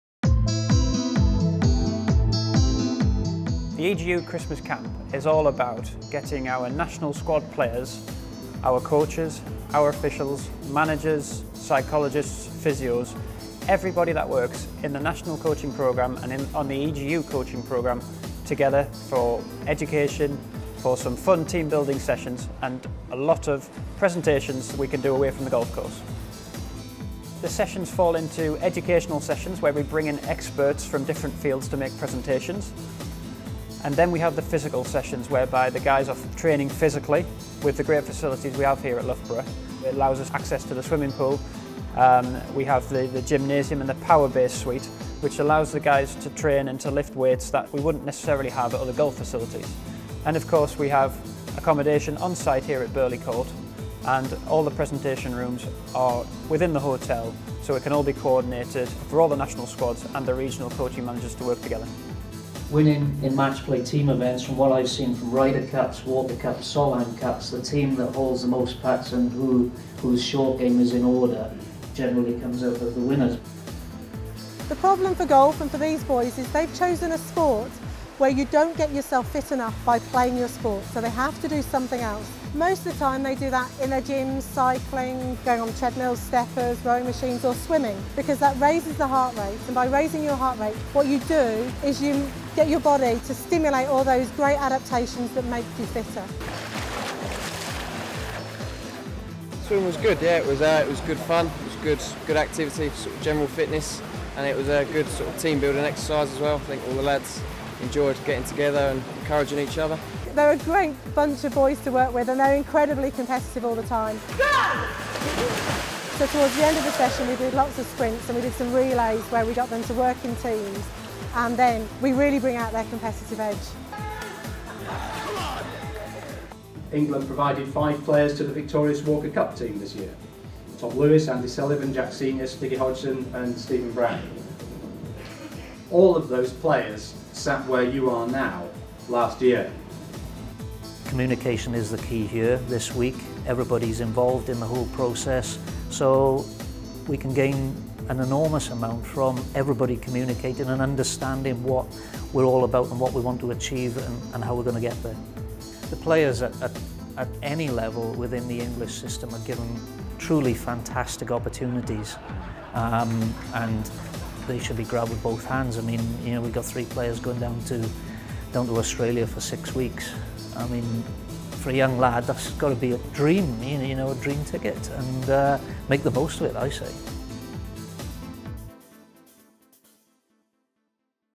Christmas Camp at Loughborough University
England squads get together for some festive team building and motivation.